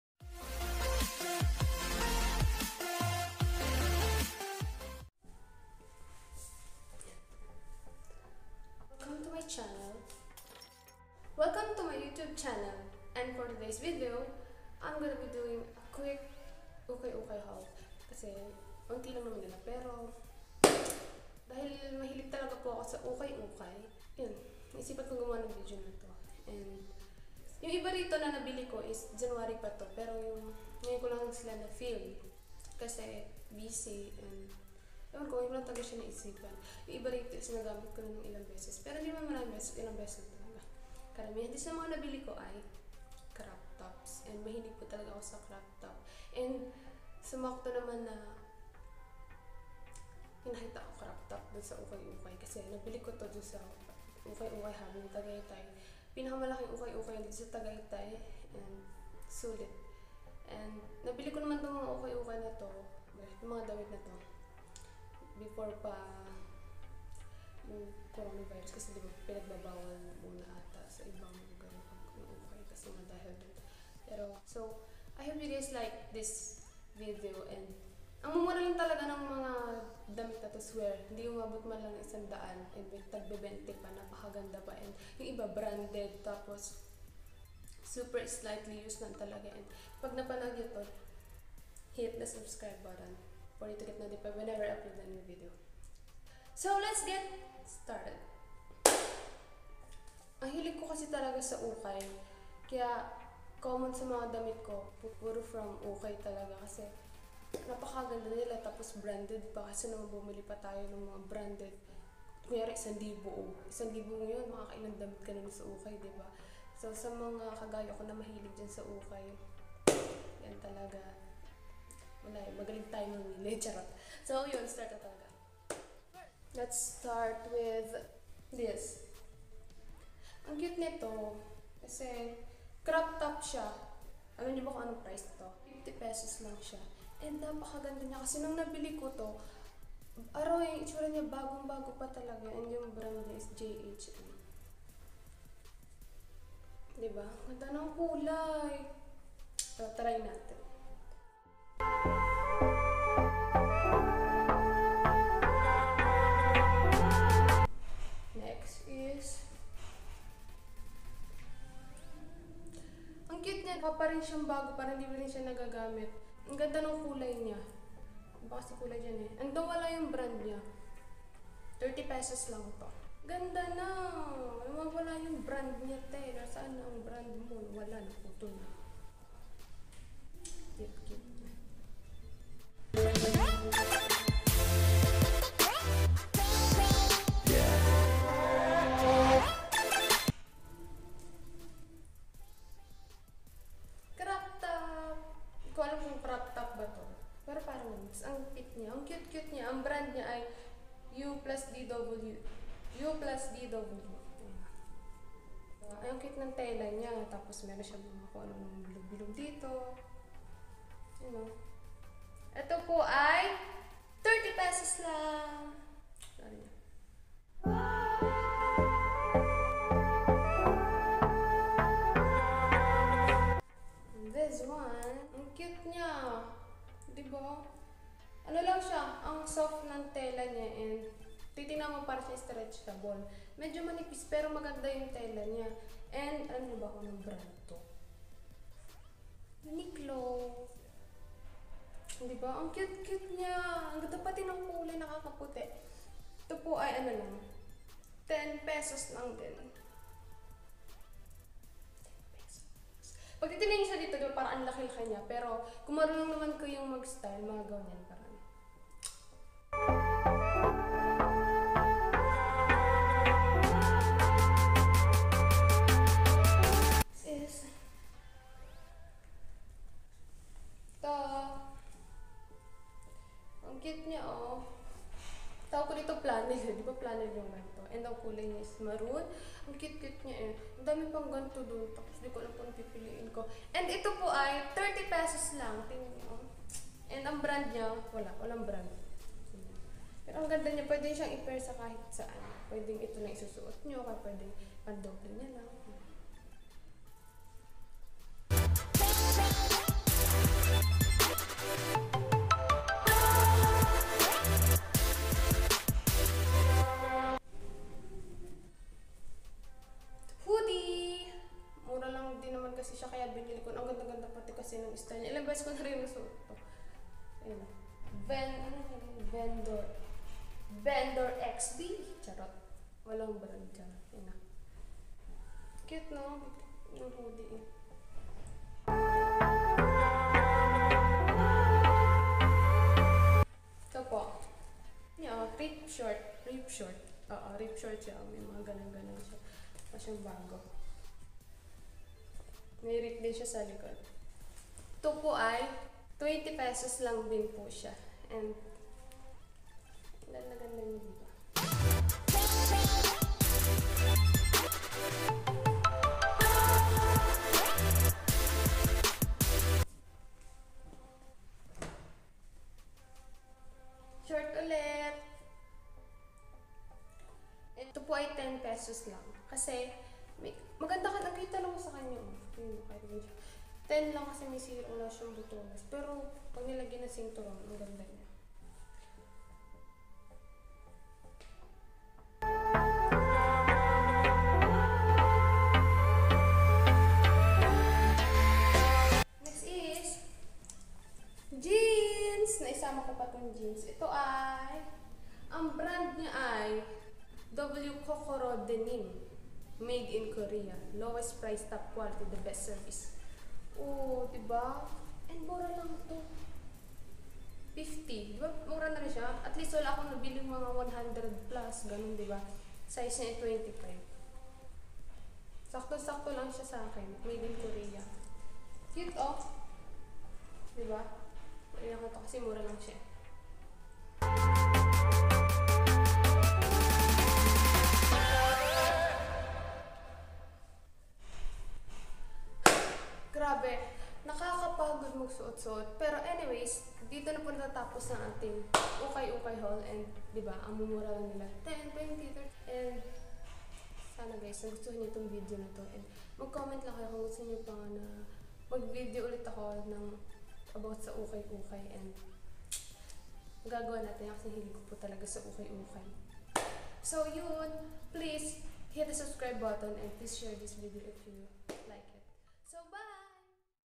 UKAY-UKAY HAUL ( TAGAYTAY ) / FOR AS LOW AS 10 PESOS | 2020 PHILIPPINES
Hindi sila ganon karami pero s'werte at nakatagpo ako ng puro crop top hahaha
But anyways, hope you enjoyed this video. See you on my next video ! ❤️